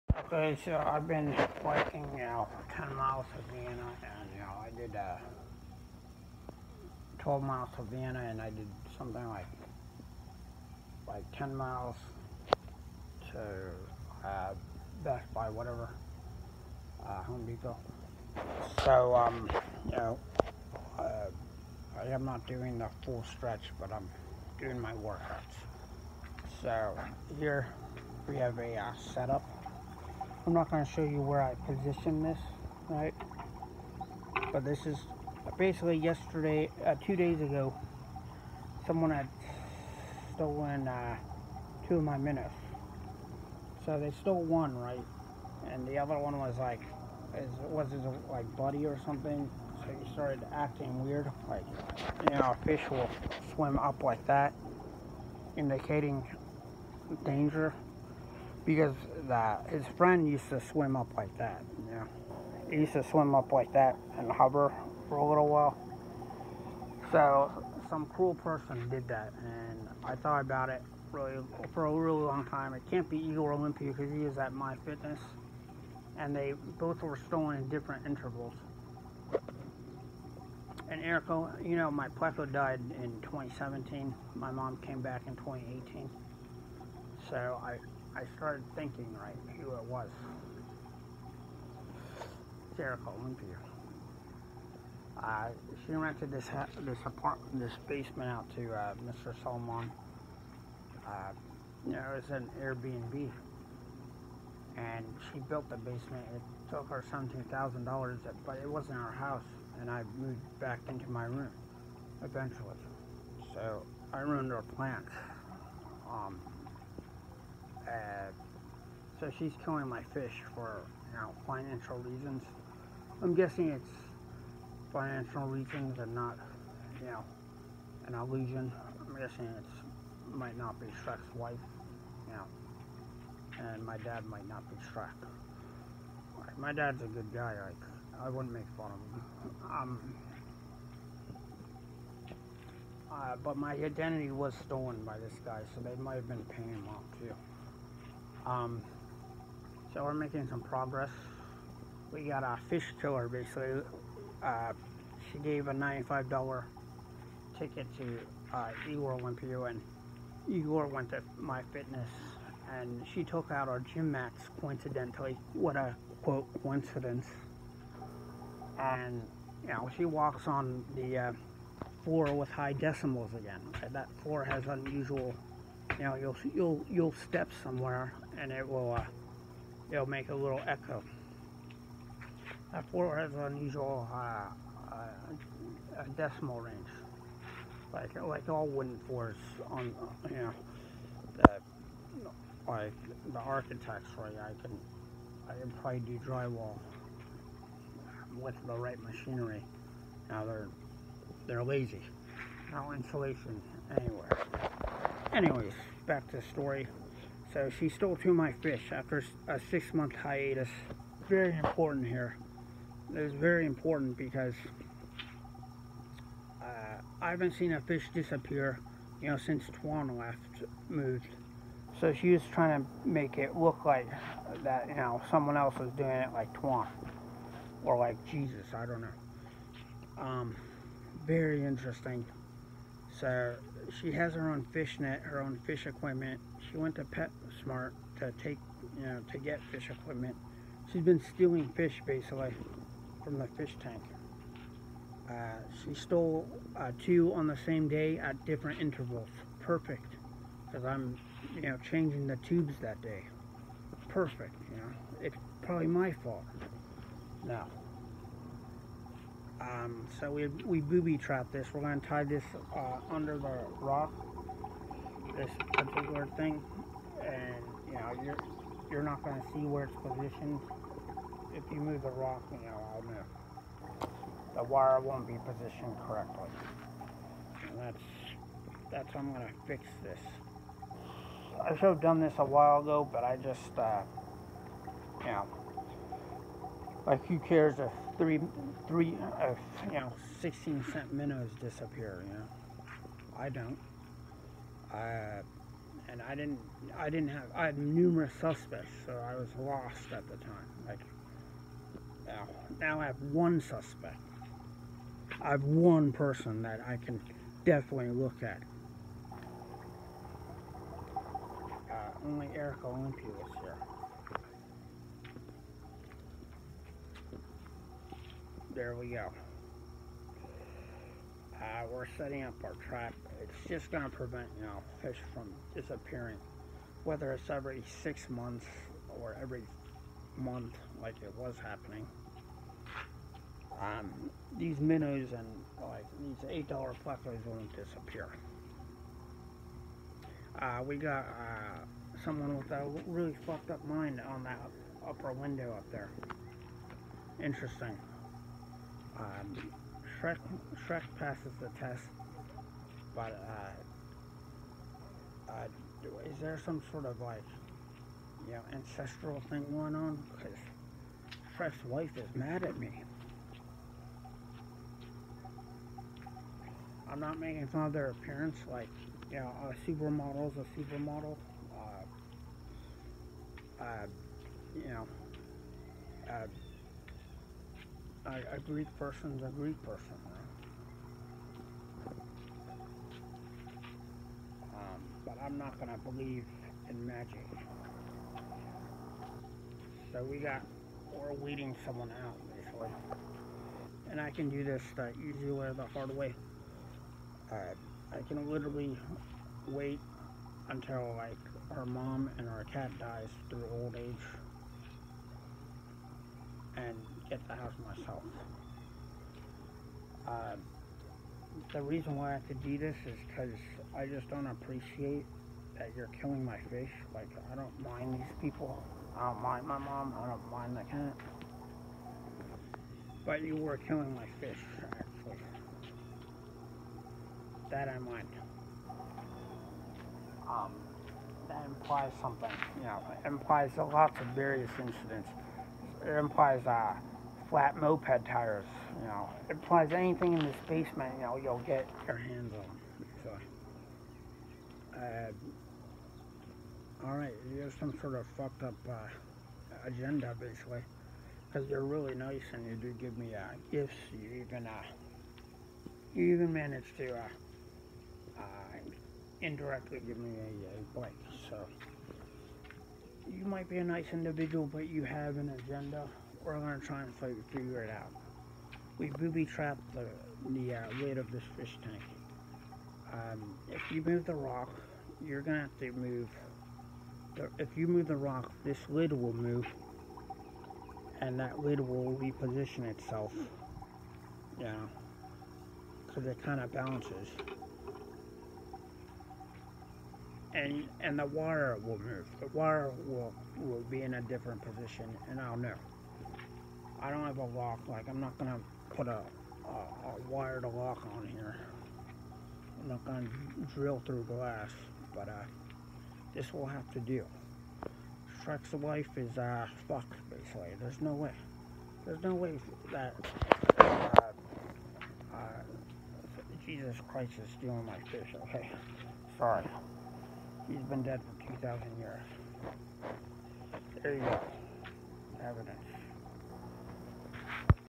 Emergency, fish stolen, shreks wife in a corner!
You’re def getting a husband in your next life and his name is going to be shrek...oh yeah and we bought new minows.  Anyways even James Bond would have a hard time w this one, I put a sliver of tape under the unseen latch where the wire is positioned.